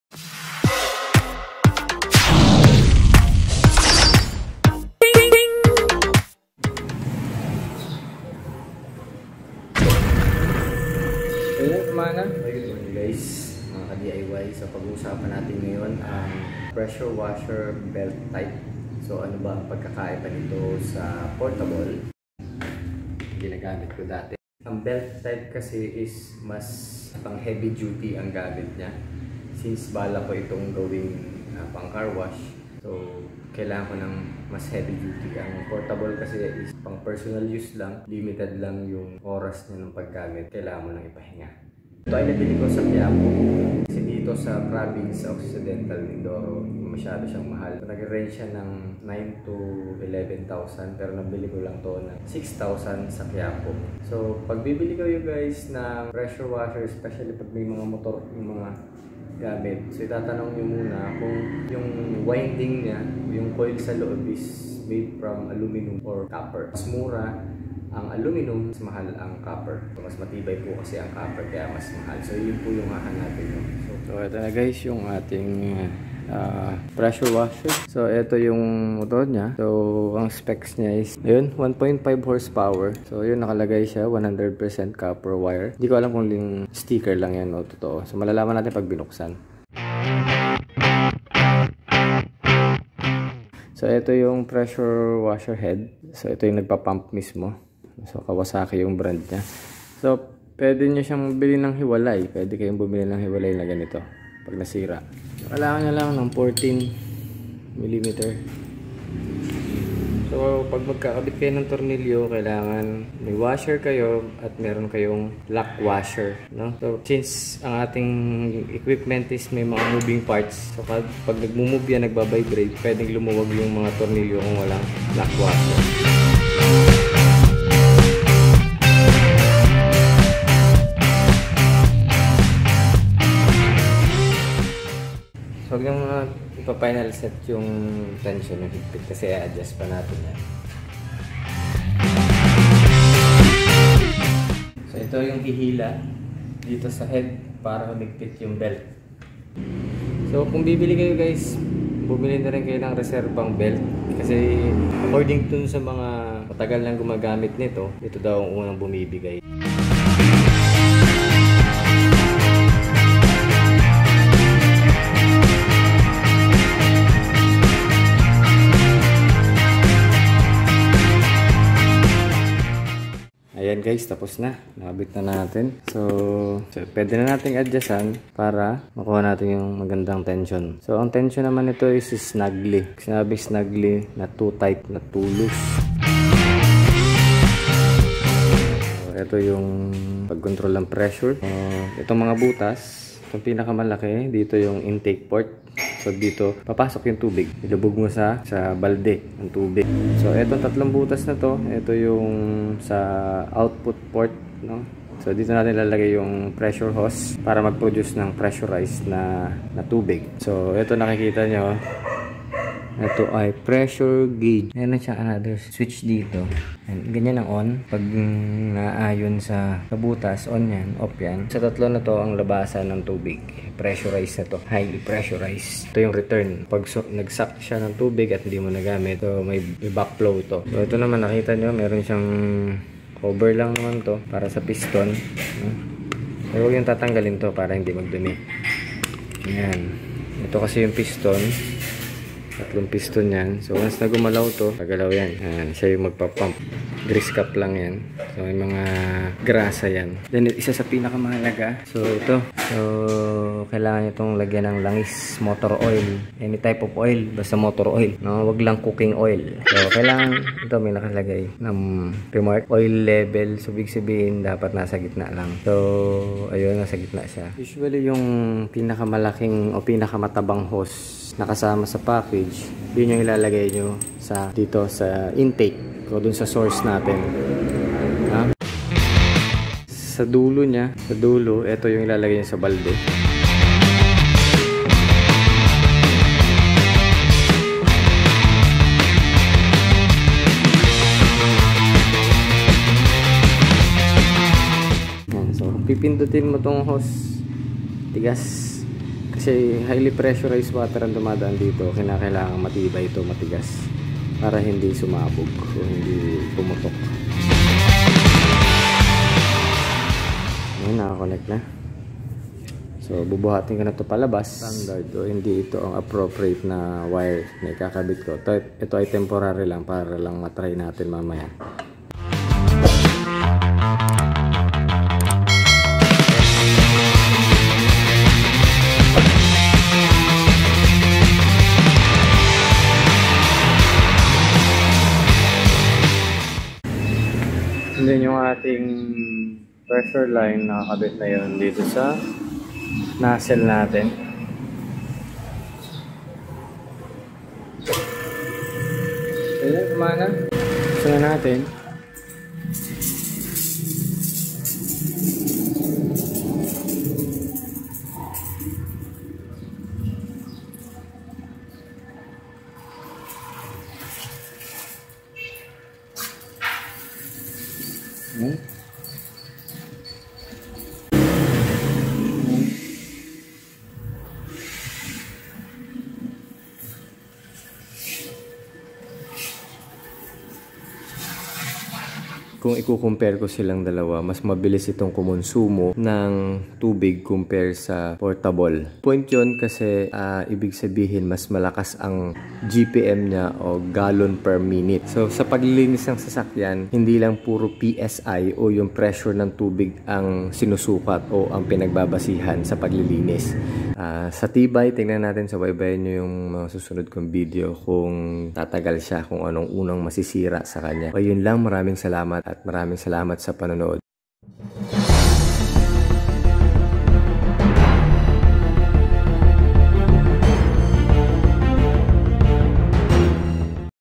Cheapest pressure washer belt type
Cheapest pressure washer belt type# Electric powered kawasaki pressure washer

 This video is about the best pressure washer belt type in the market. It provide unboxing video, basic assembly procedure, testing and review to the pressure washer belt type. It also discussed important parts and function as well as simple comparison to portable car washer. The price and variant of pressure washer belt type was also revealed.

skip Intro 00:07
Pressure washer belt type vs portable 0:24
Price of pressure washer 0:53
Copper vs aluminum winding 1:04
How to assemble Pressure washer 1:15
Unboxing pressure washer head (kawasaki) 1:37
Parts and function of pressure washer 3:10
Wiring 5:17
Initial testing 5:36
Setting pressure and angle 5:55
Conclusion 6:25

Add: San Jose, Occidental Mindoro